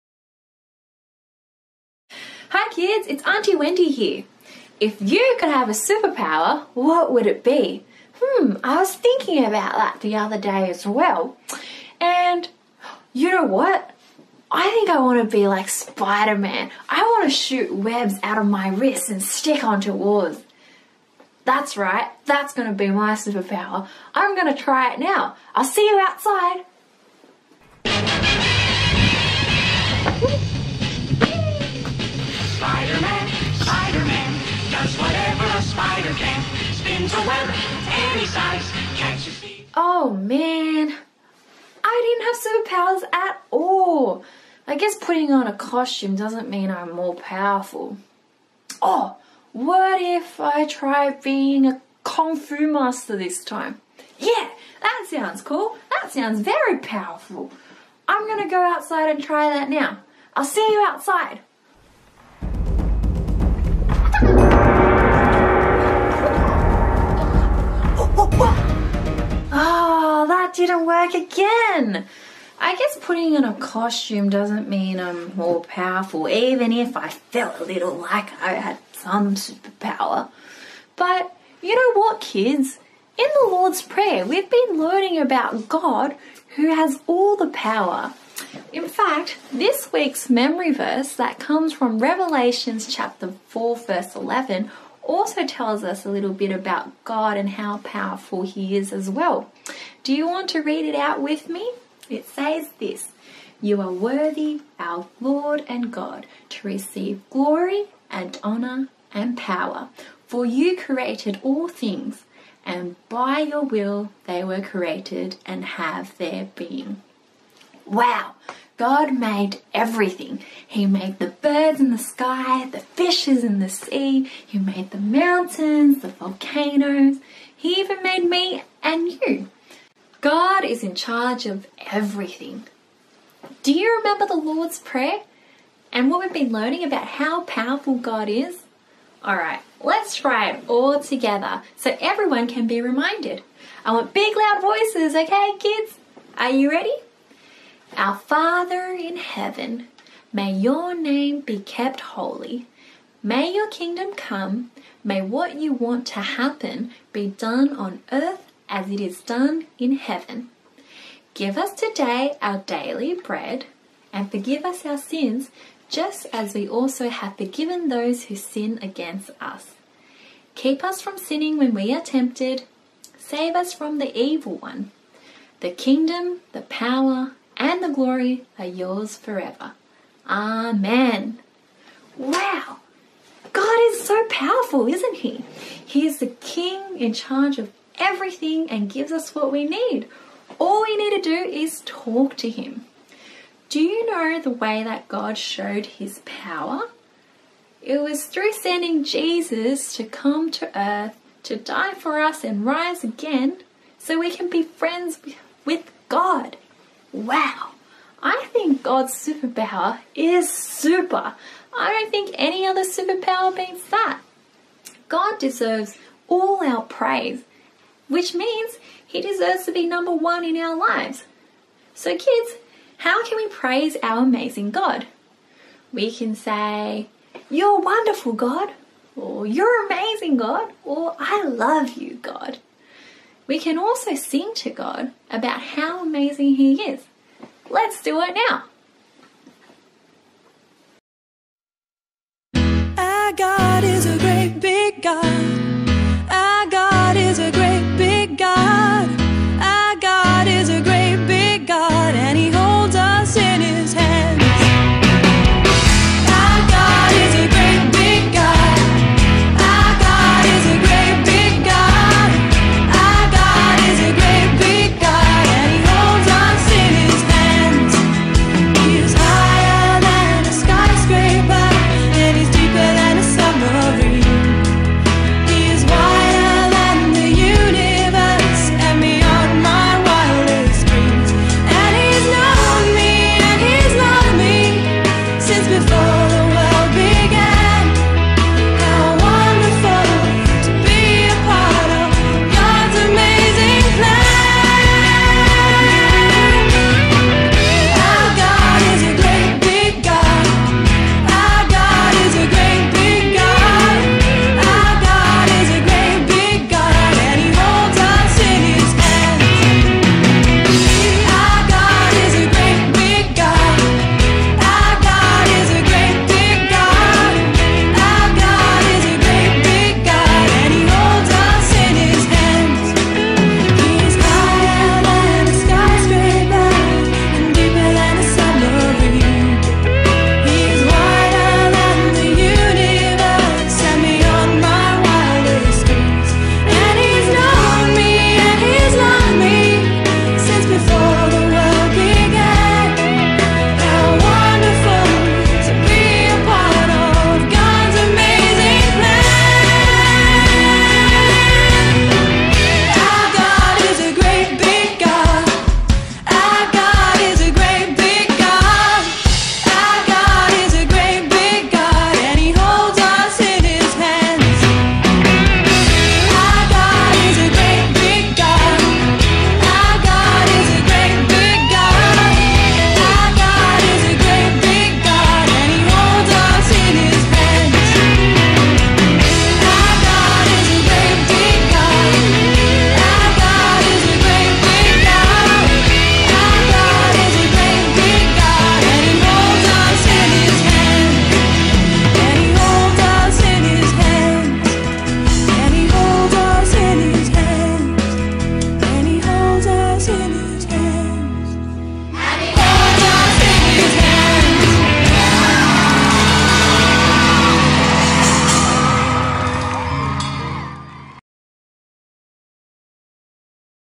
2020-09-13 Kids Talk
Aunty Wendy talks to the kids about the last line of the Lord's Prayer: "The kingdom, the power, and the glory are yours forever. Amen"